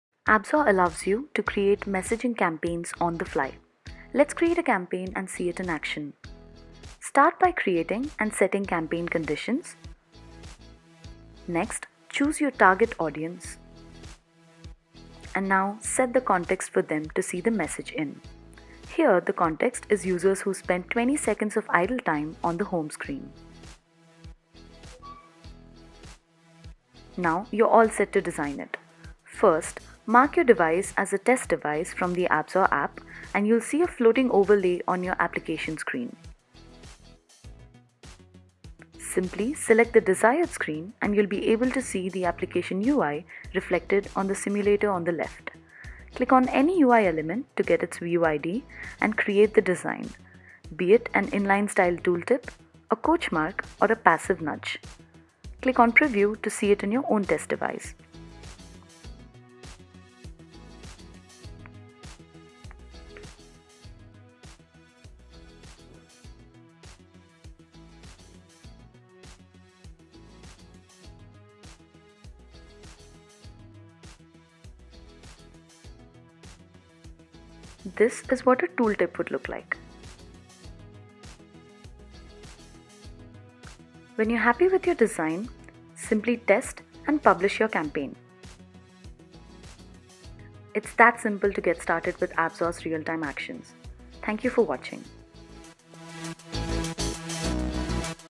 Real Time Action Toolkit: Creating campaigns is now easier than ever. | Apxor
With Apxor's advanced messaging capabilities, create campaigns effortlessly. Walkthroughs, inline and in app messages are easy to create and test on your own devices. What you see is what you get!